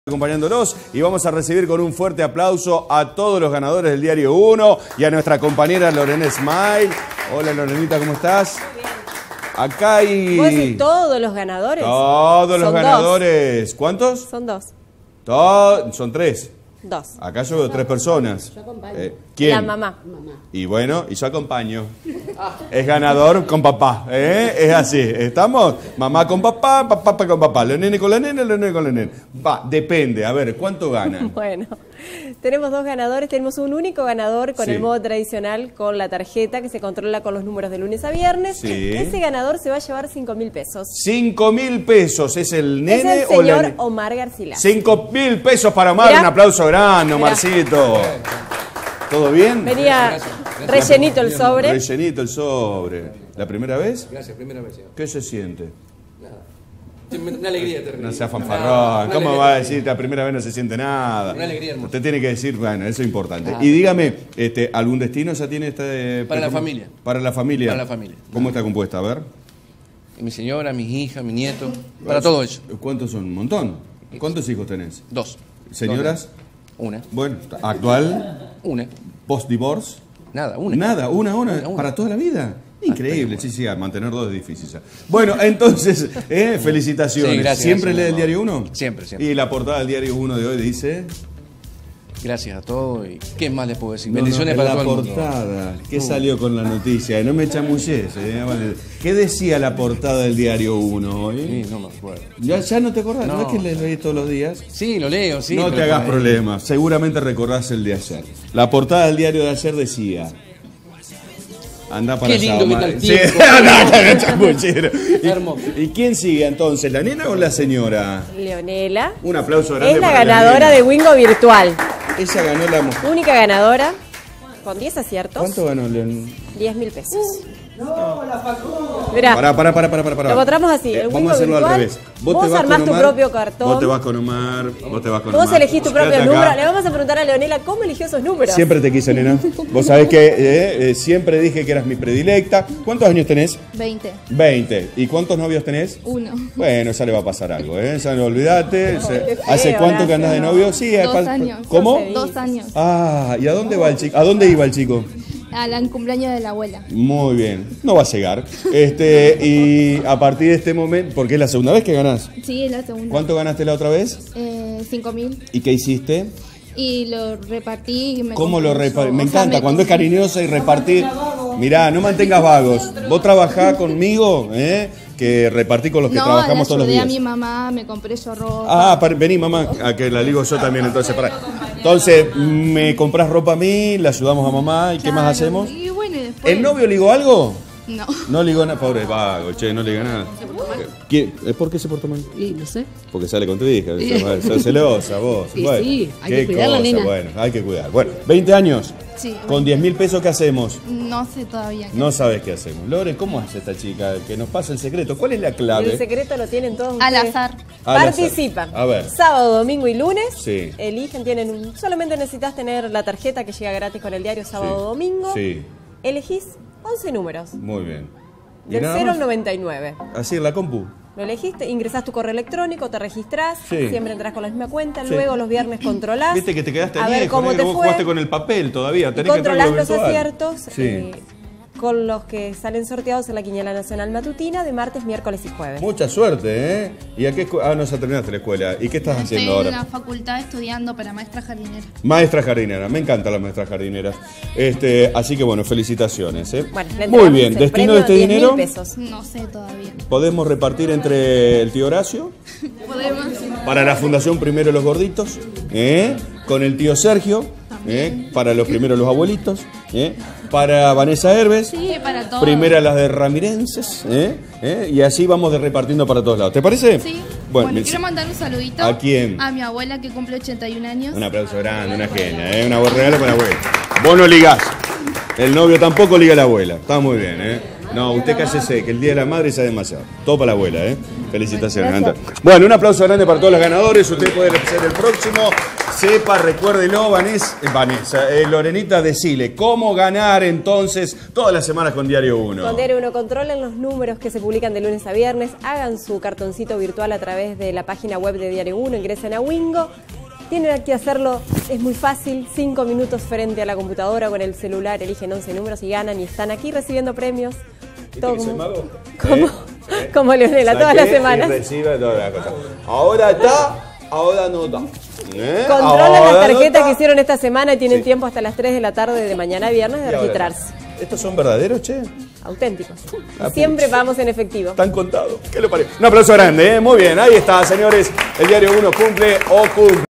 Dos para recibir
En la modalidad virtual y la normal hubo un ganador respectivamente. Conocemos acá a felices ganadores del wingo